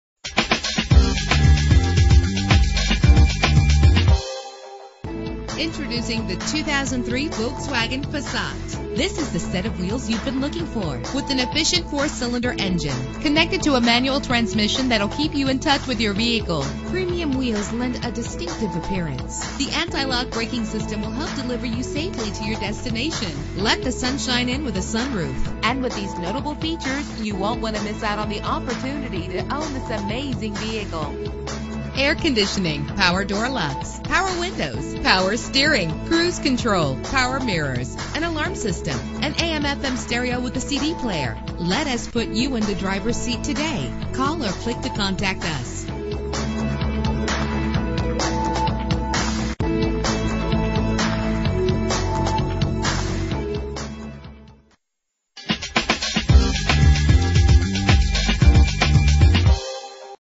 2003 Volkswagen Passat - Leesburg FL
Year: 2003
Make: Volkswagen
Model: Passat
Trim: GLS
Engine: 1.8 liter inline 4 cylinder 20 valve
Transmission: 5-Speed Manual
Color: Gray
Mileage: 86477
Address:
8629 US Hwy 441